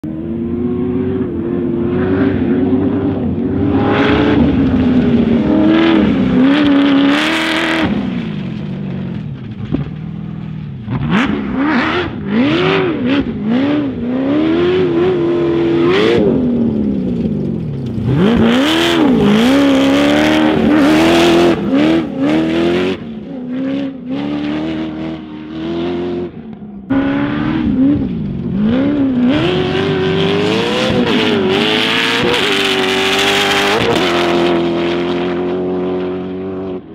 Garrett George Getting Down in a Funco Sand Car in Glamis
In this short video, Garrett does some turns and wheelies in a brand new Funco sand rail. Listen to the sound of the LS Engine!

Watch this sand buggy wheelie in Glamis in this video.